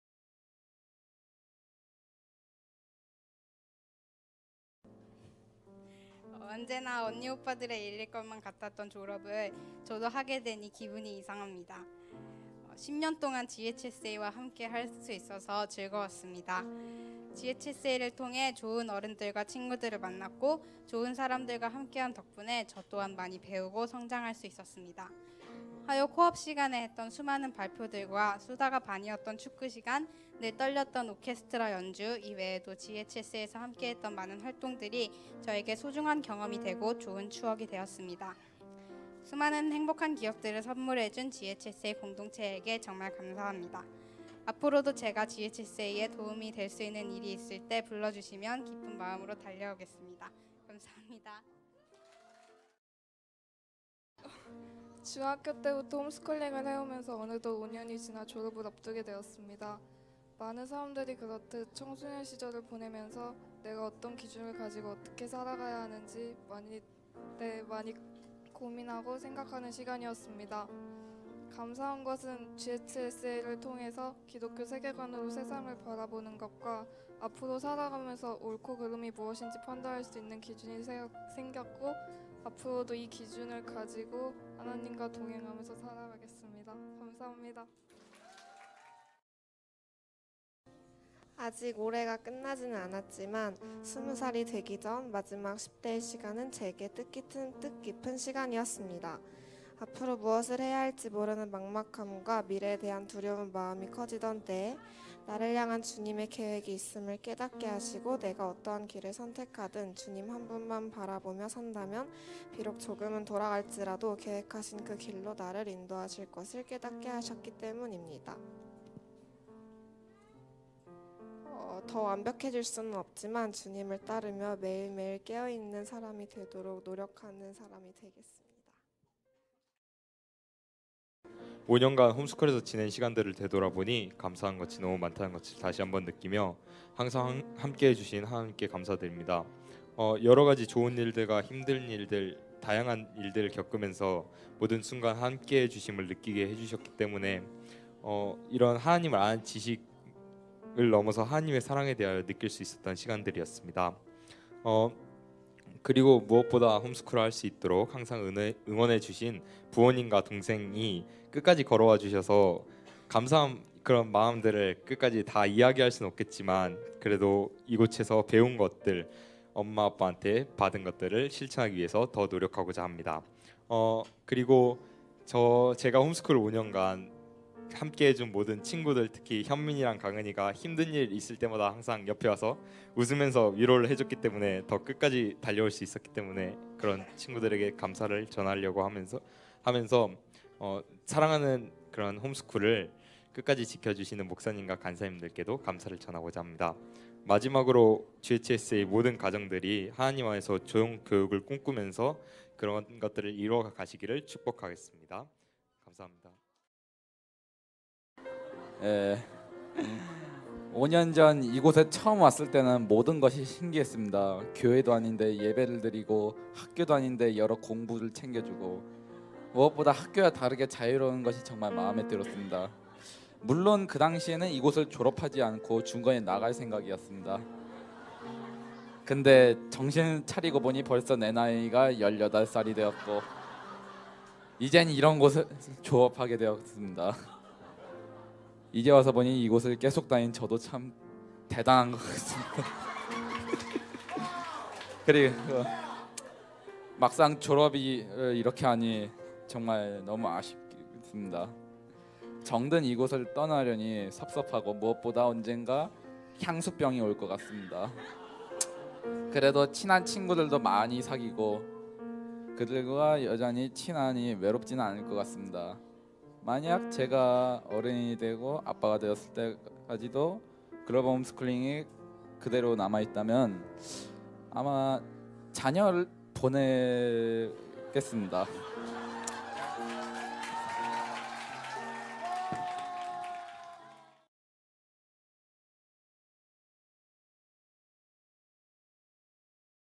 2018 졸업식 졸업생 소감발표 최린 김채연 남우림 김유빈 송강은
2018 졸업식 졸업생 소감발표
졸업자 : 최린 김채연 남우림 김유빈 송강은